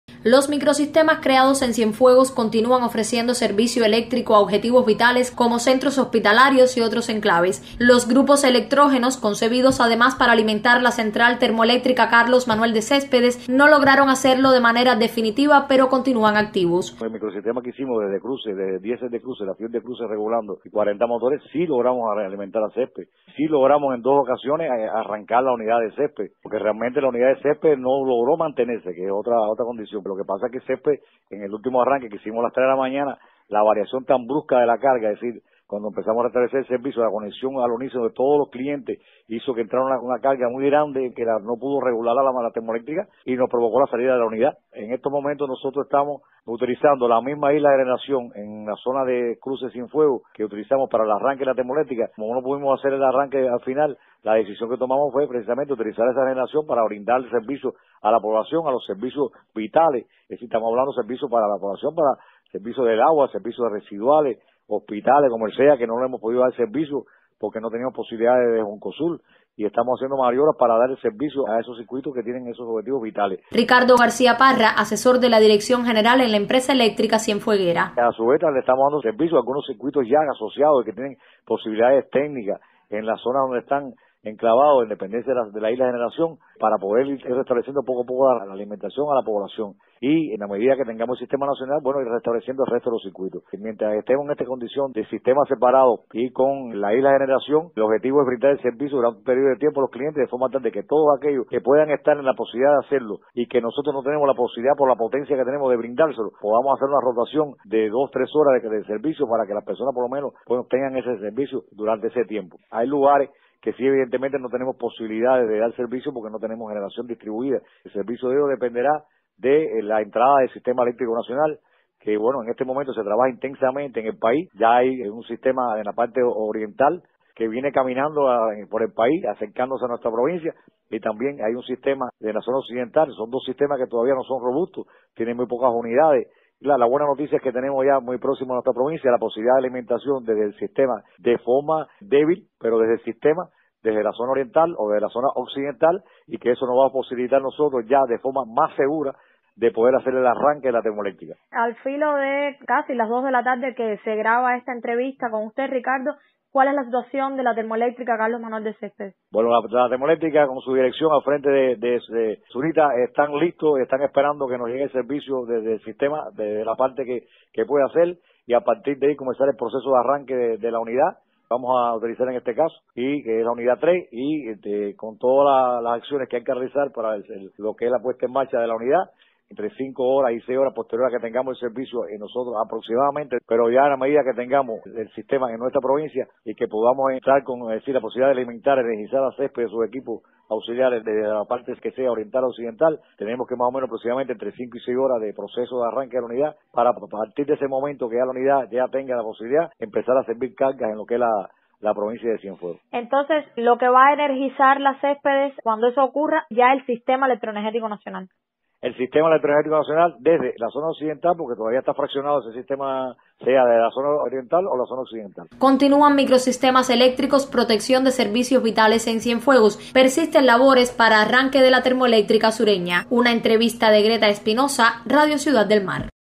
Situación energética en Cienfuegos en la tarde de hoy 29 de septiembre
Autoridades de la Empresa Eléctrica en Cienfuegos actualizan sobre las labores para el arranque de la Central Termoeléctrica Carlos Manuel de Céspedes, y la protección mediante microsistemas eléctricos de servicios vitales en Cienfuegos. Escuche las declaraciones de Ricardo García Parra, asesor de la Dirección General en la Empresa Eléctrica sureña.